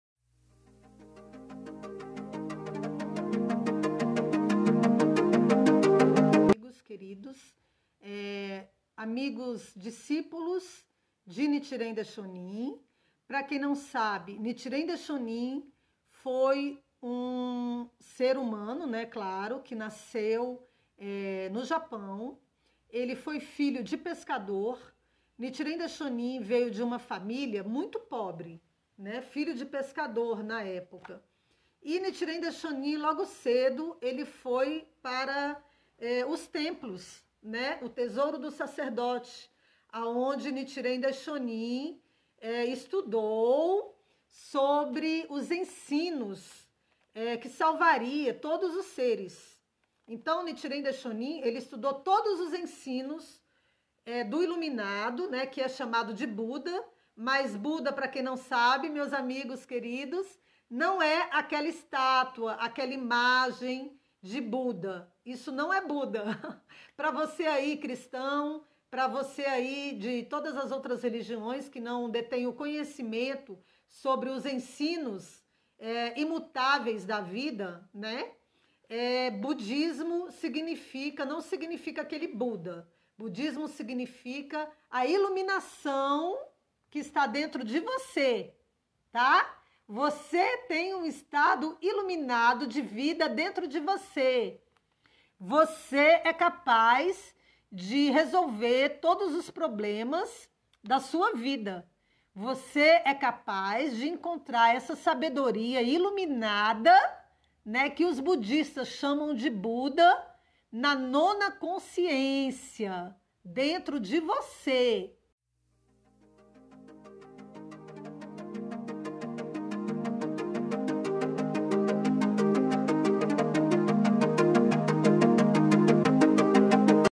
O que é Buda
Nitiren Daishonin se iluminou para as verdades imútaveis da vida . Nasceu como filho de pescador e veio de uma família muito pobre humilde e desejou ser o homem mais sábio de todo o Japão. Nitiren Daishonin estudou todos os ensinos do Iluminado Sakyamuni .